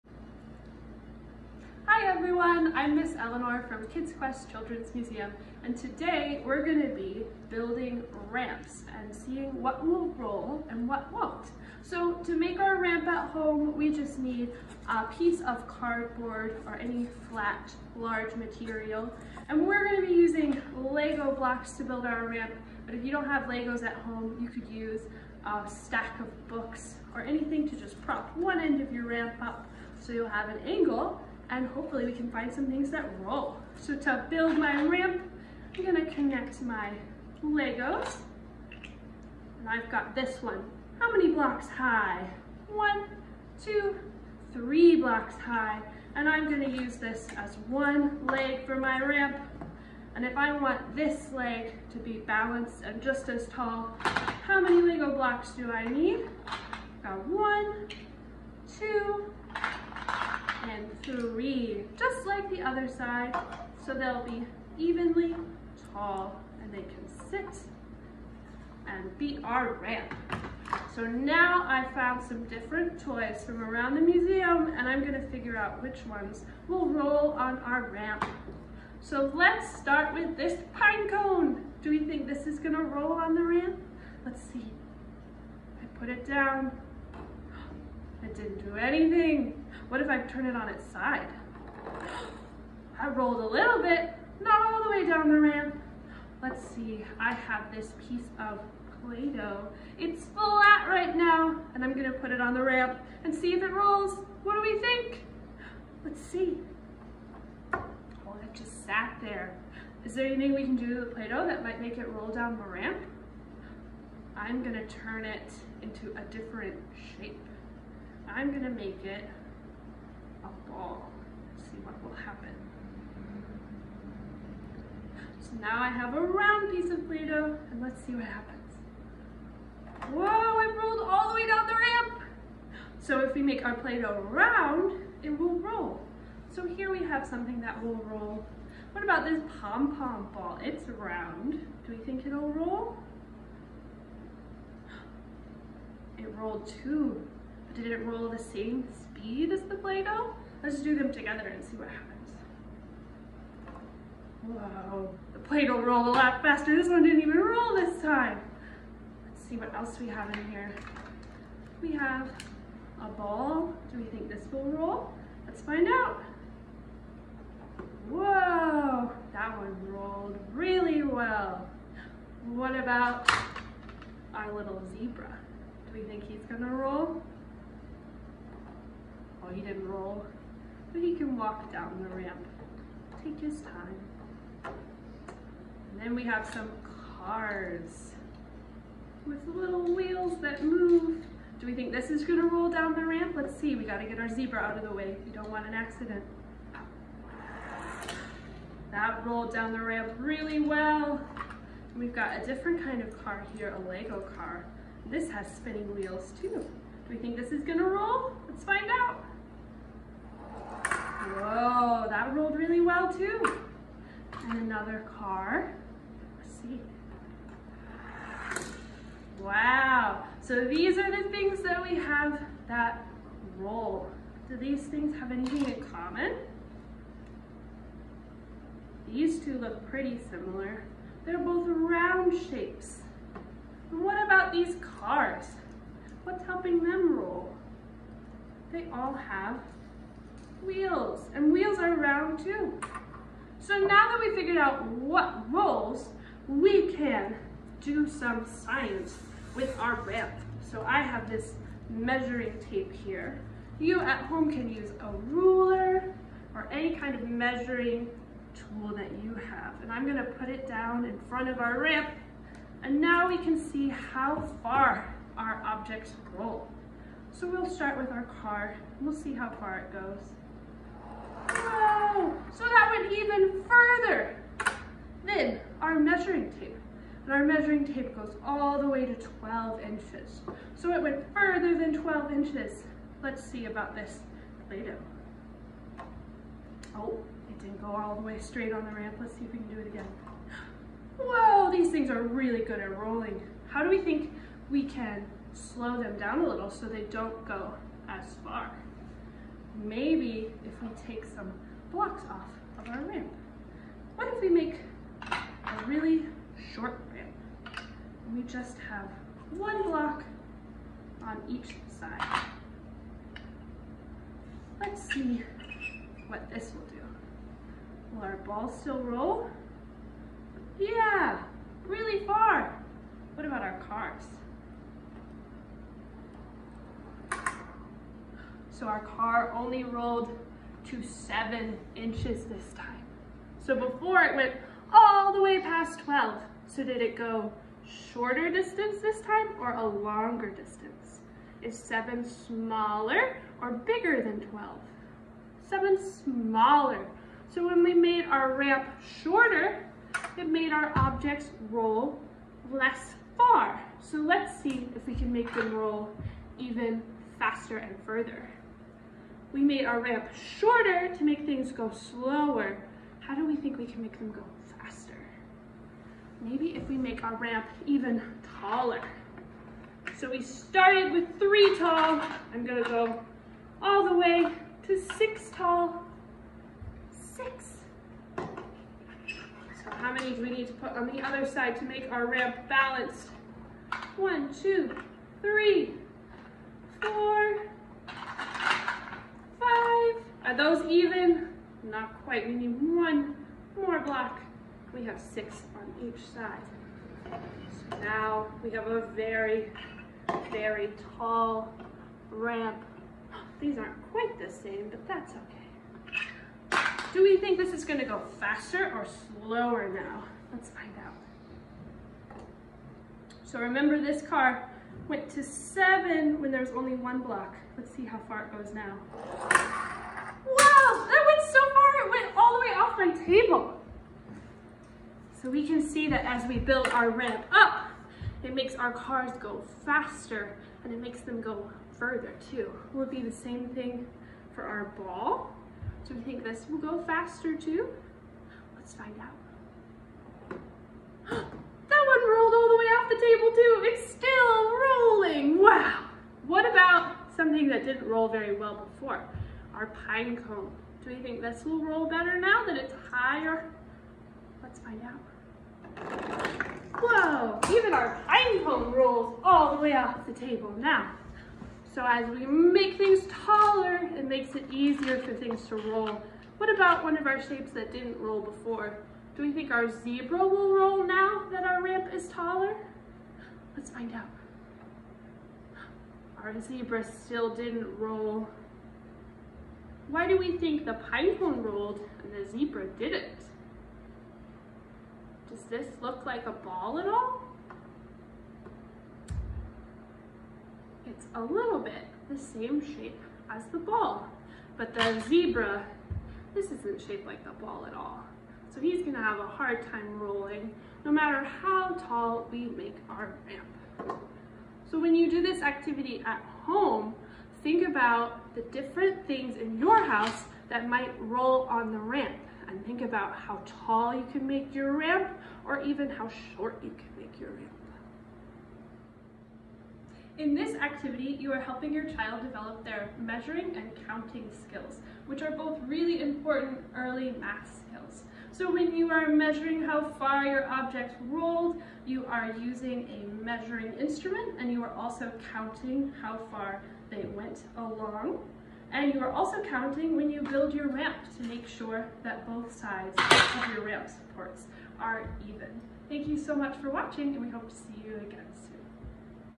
Ramp Activity: Measuring and Counting
Follow along with Eleanor as she’ll show kids how to set up a ram and measure the distance each objects go. Younger kids will love testing different object and older kids will work an their Early Math Skills as they calculate distance and practice measuring.